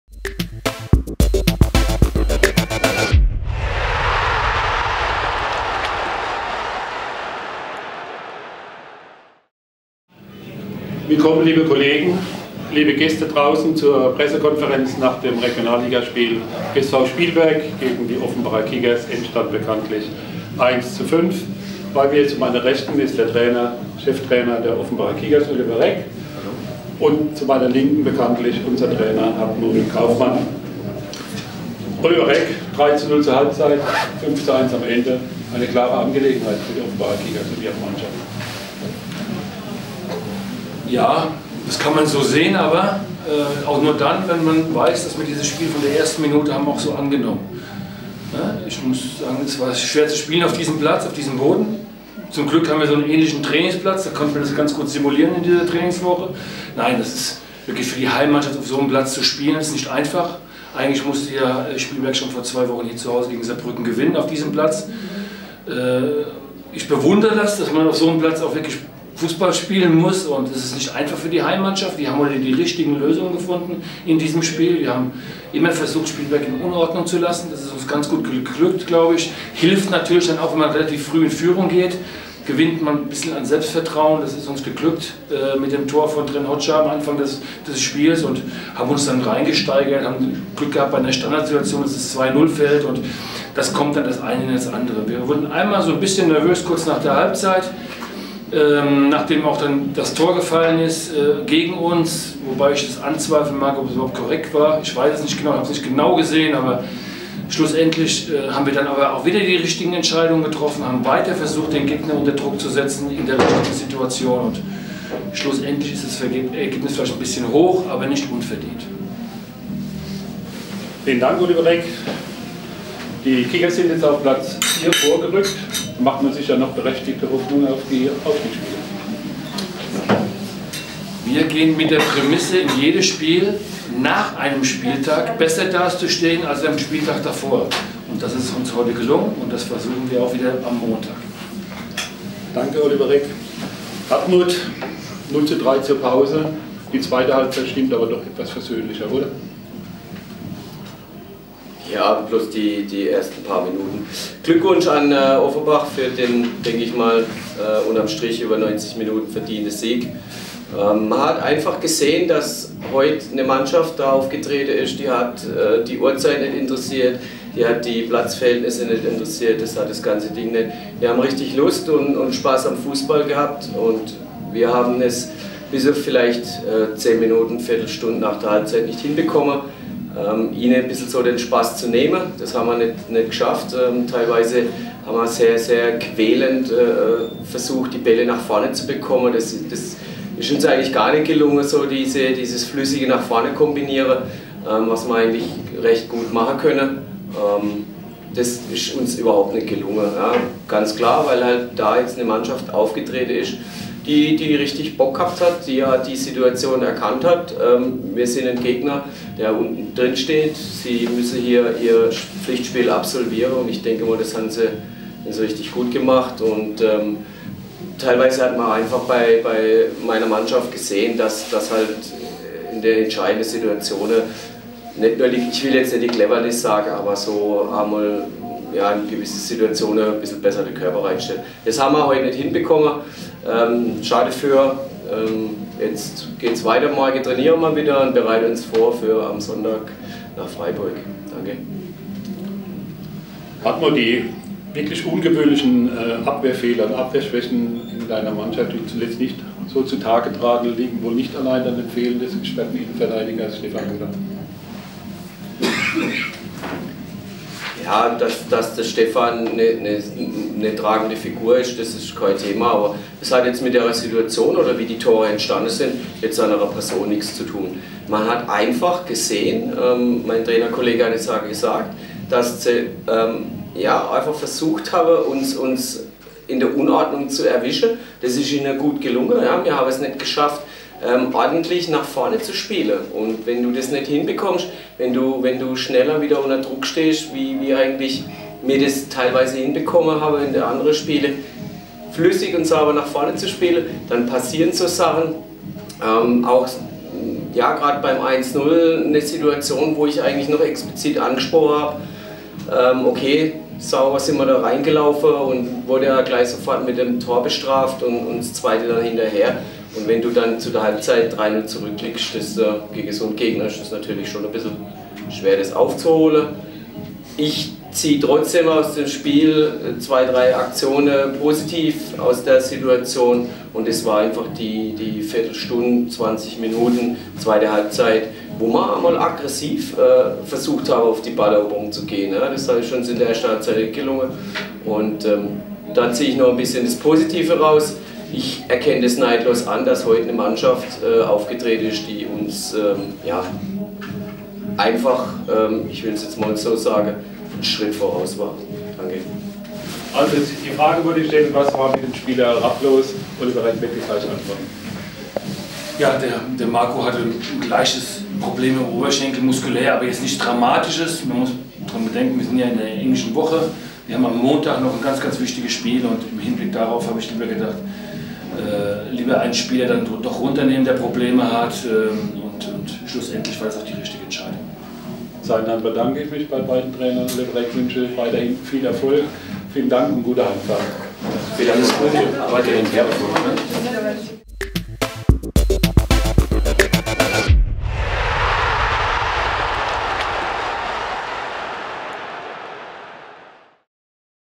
Pressekonferenz nach SV Spielberg - Kickers Offenbach vom 23.3.2016
Stimmen der Trainer Oliver Reck (Kickers Offenbach) und Hartmut Kaufmann (SV Spielberg) zur Partie SV Spielberg vs. Kickers Offenbach am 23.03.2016, dem 24. Spieltag der Regionalliga Südwest 2015/16.